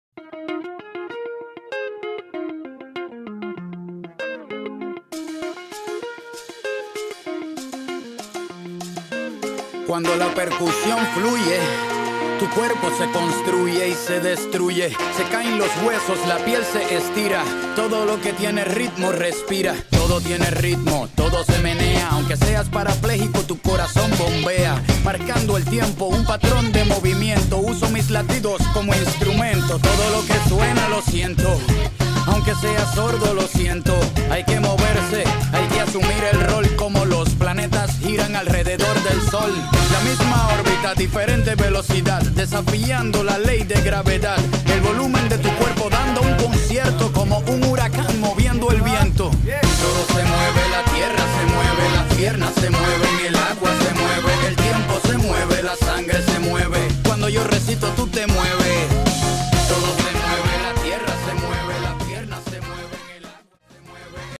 Mas amor, menos motor (mural)
Arte mural realizado par la Urb. El Charaguamal en Valencia.